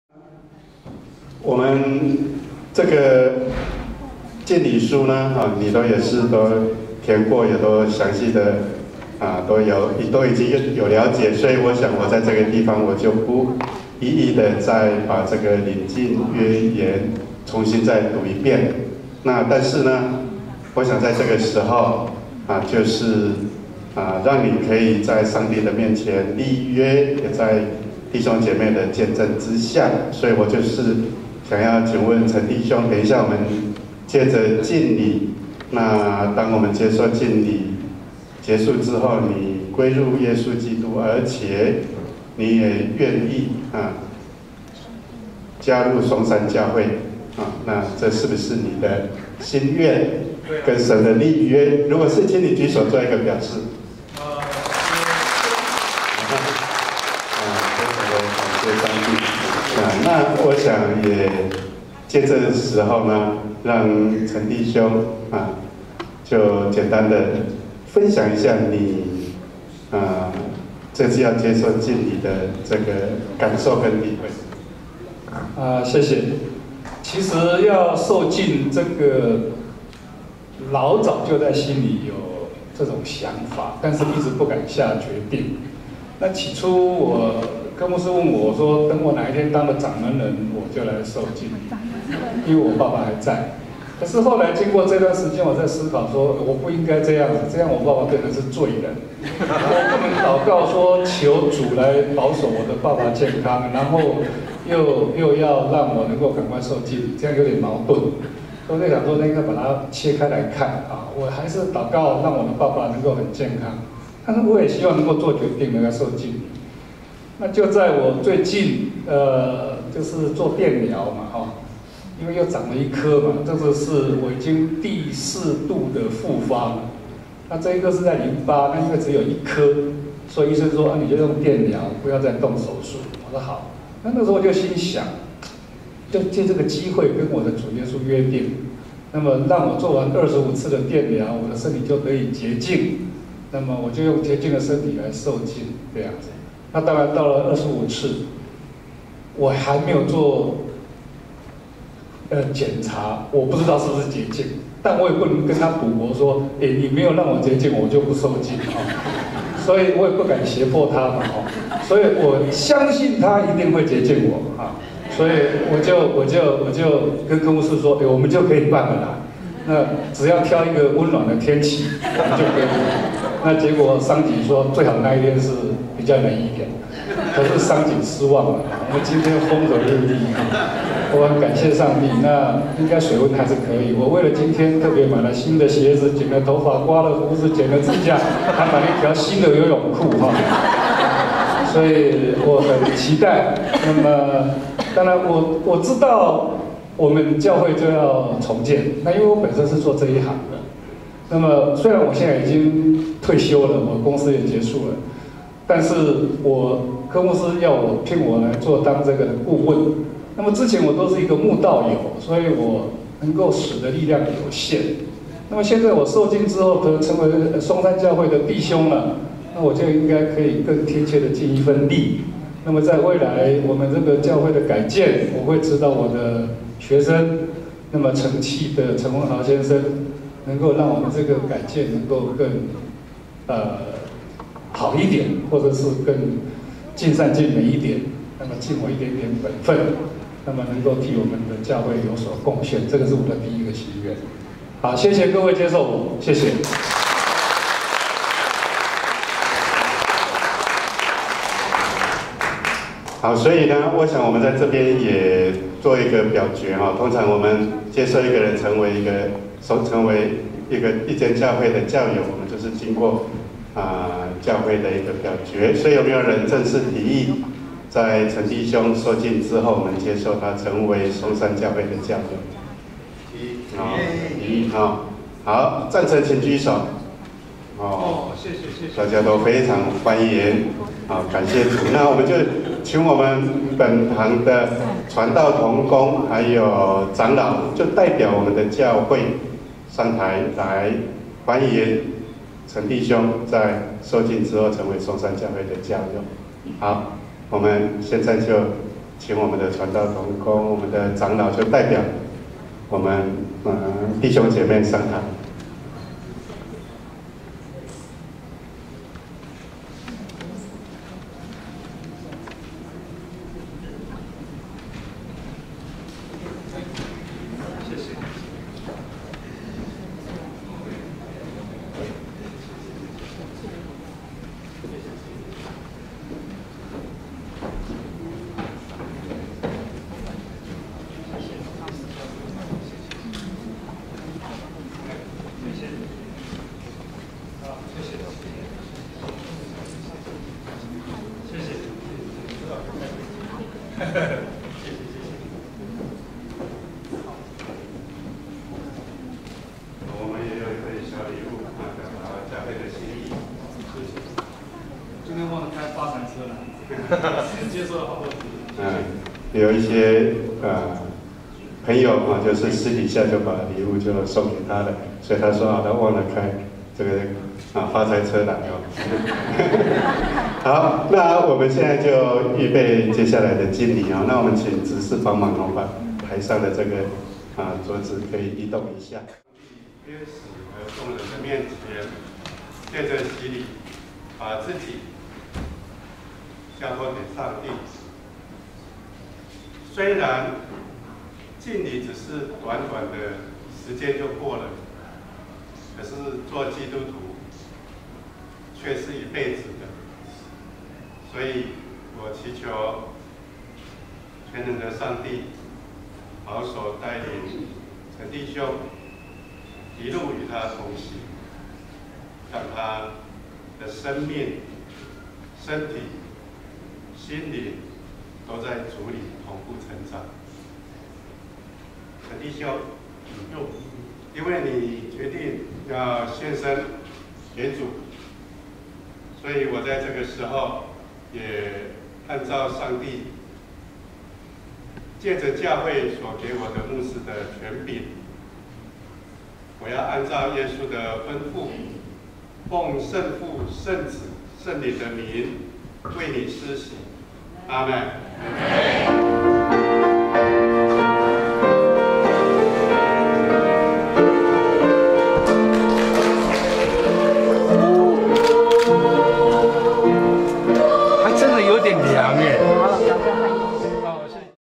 2013-03-16陳德貴弟兄浸禮
陳德貴弟兄因為參加臺安醫院的新起點健康生活營，有機會認識上帝，近二年來，他殷勤查經，努力追求，因著上帝的恩典和呼召，陳弟兄今日受浸歸主，作主門徒，願全能的上帝賜福陳弟兄全家。